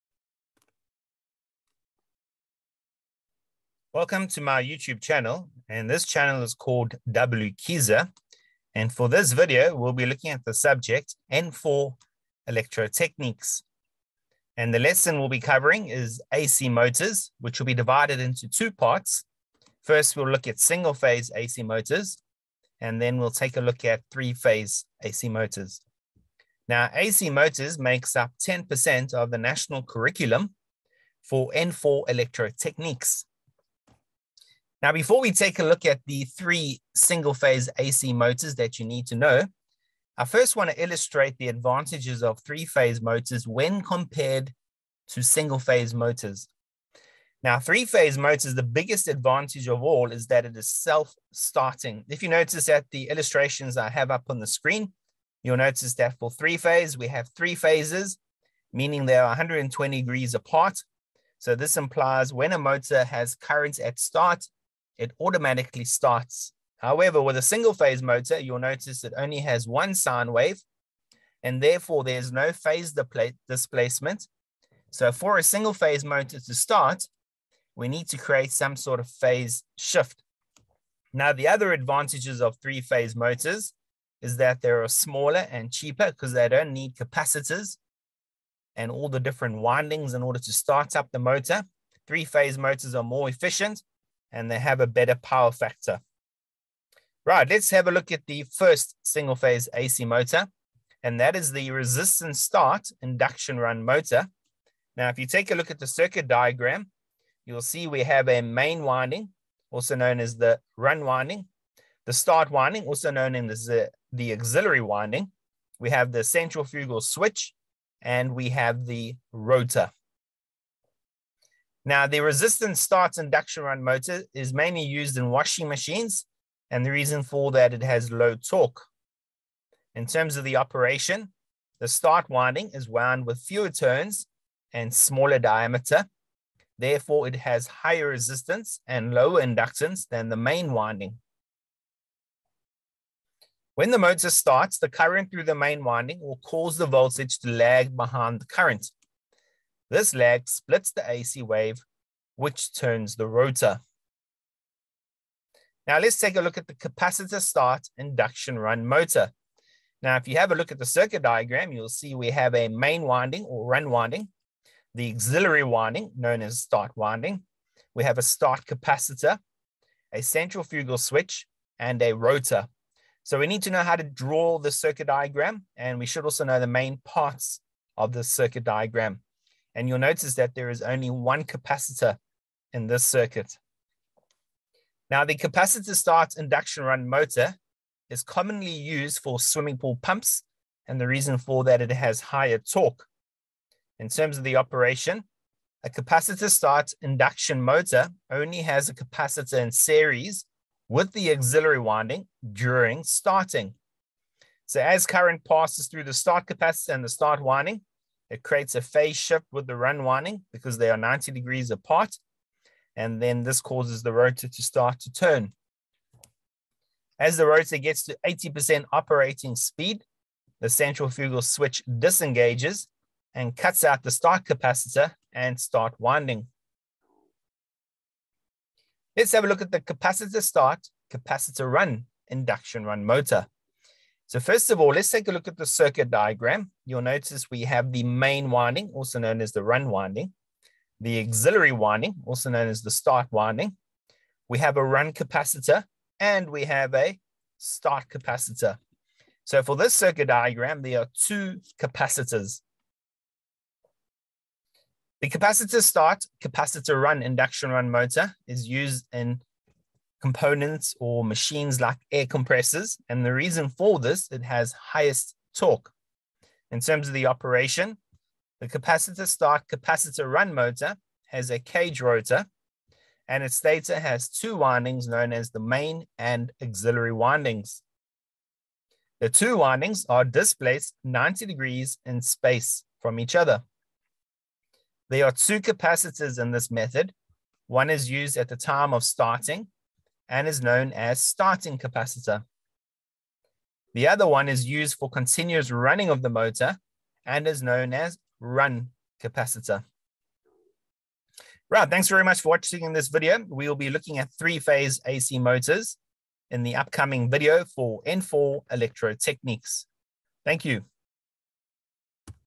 Single Phase AC Motors
N4 Electrotechnics lesson on Single Phase AC Motors